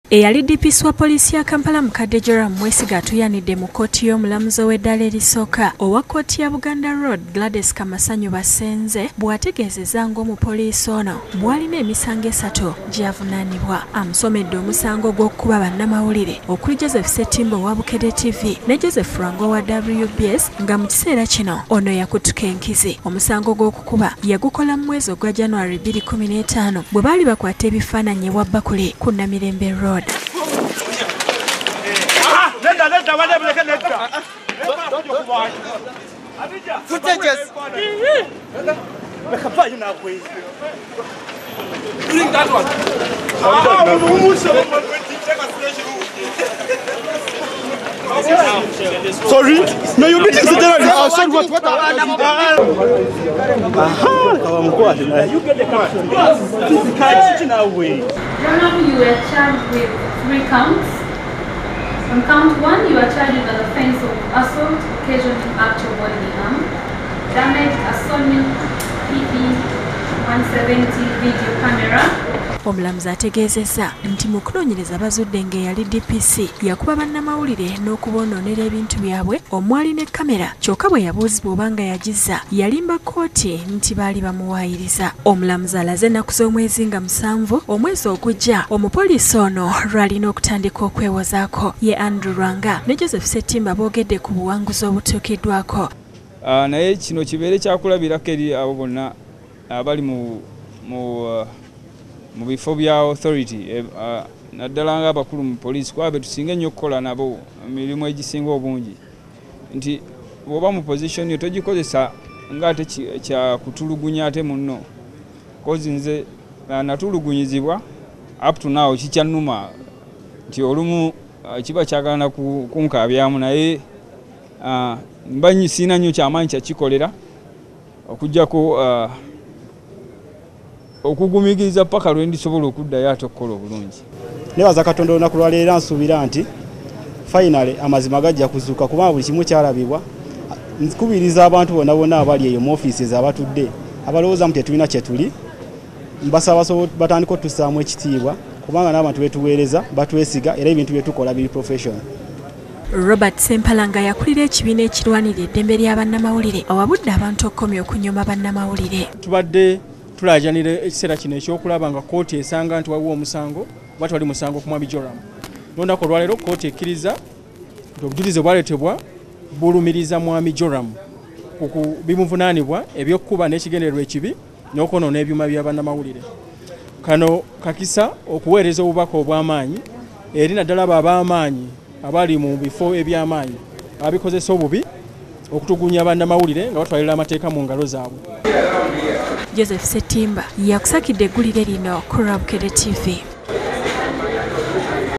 Eyali DPC wa Kampala mukadde wakwewozaako
Yeenyigira mu kukuba bannamawulire.